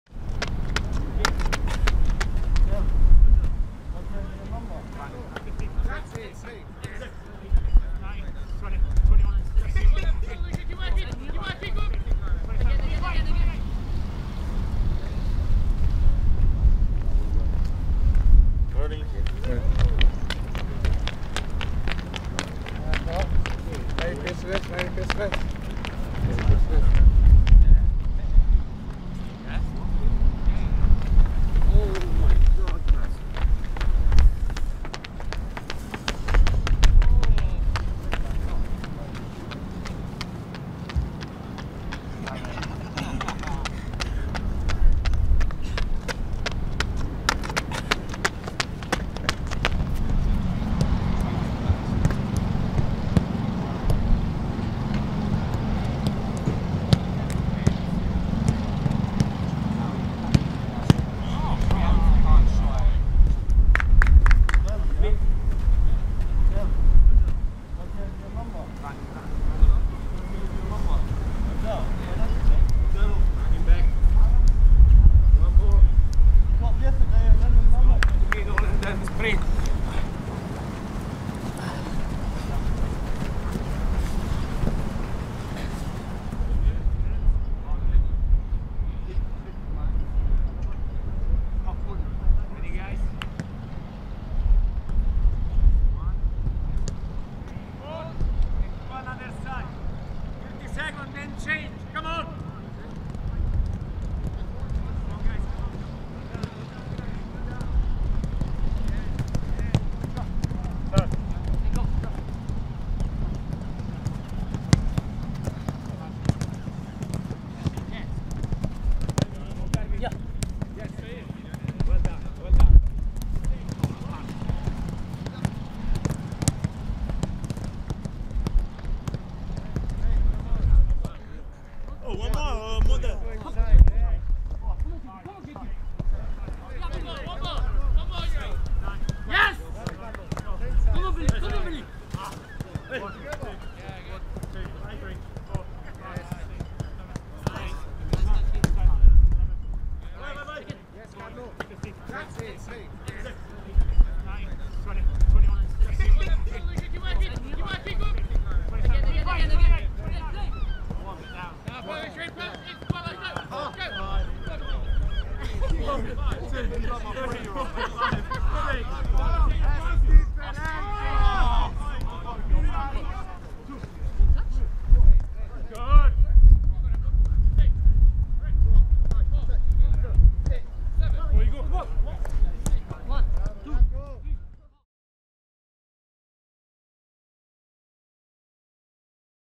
Rondos & Estupinan's Return! 🇪🇨🤩 | Brighton's Inside Training
Ahead of facing Tottenham, Pervis Estupinan rejoins the group and De Zerbi's side look ahead to the clash tomorrow night.